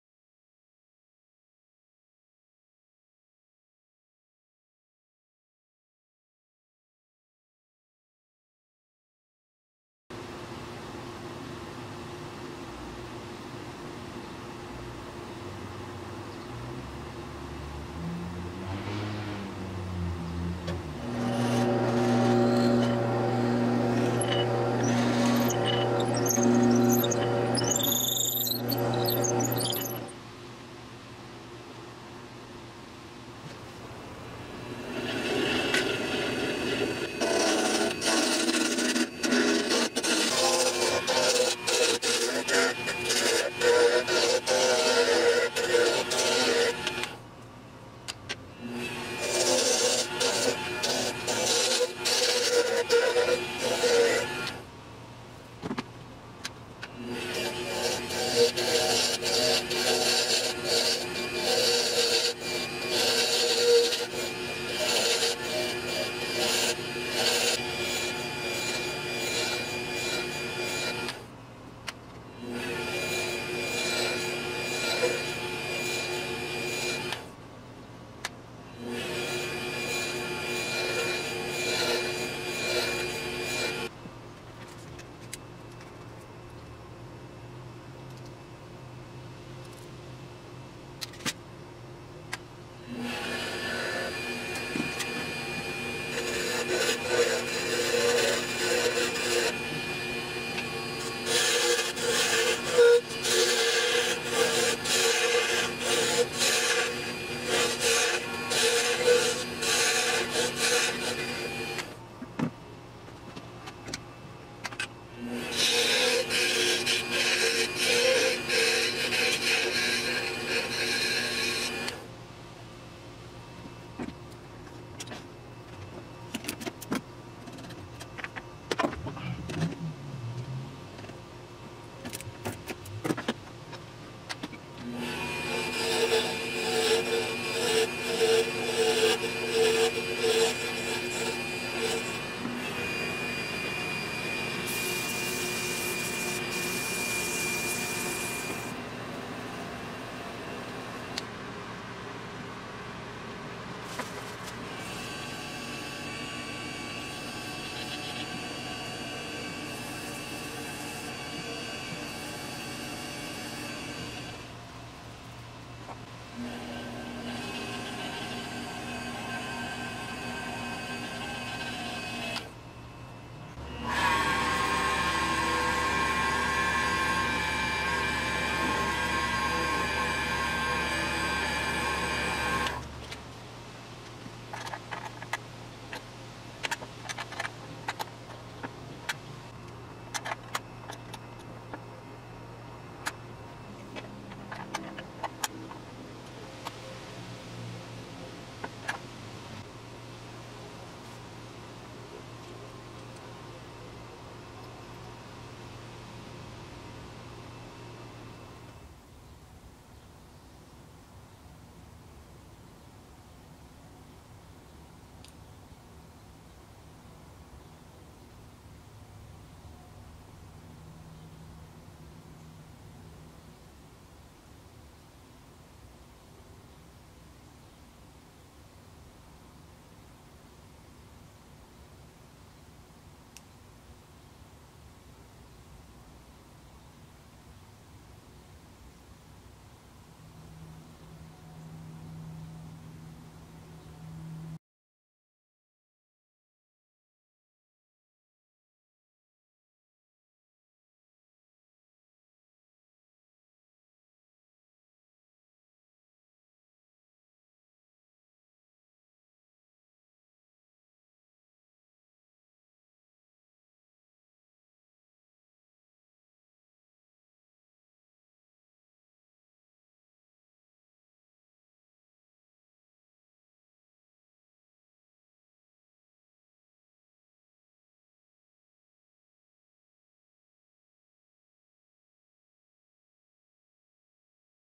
50 Cal Cartridge Twist Pen (With Buckeye Burl)
In this video watch as I turn a piece of burl wood into a 50 Caliber Cartridge twist styled pen. Want to make an order?

Faith Hope Love Pen Kit

CA Glue

Carnauba Wax